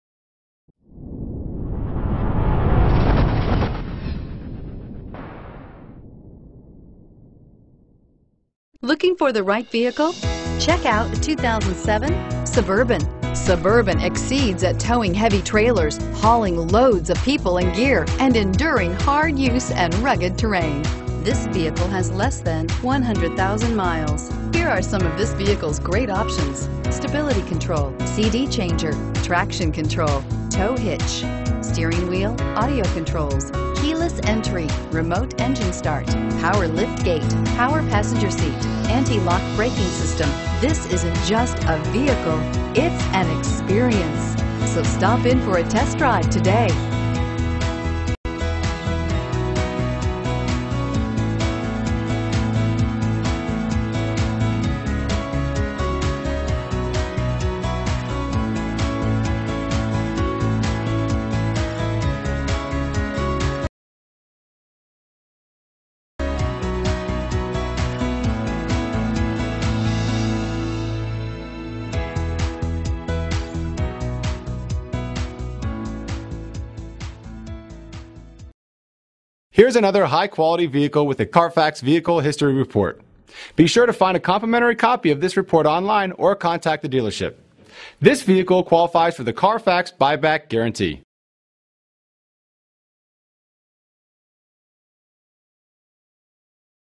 2007 Chevrolet Suburban Metairie LA PRH228757
2007 Chevrolet Suburban LTZ

Pick up this Black 2007 Chevrolet Suburban, available today at Hyundai of Metairie. This could be the one you've been searching for.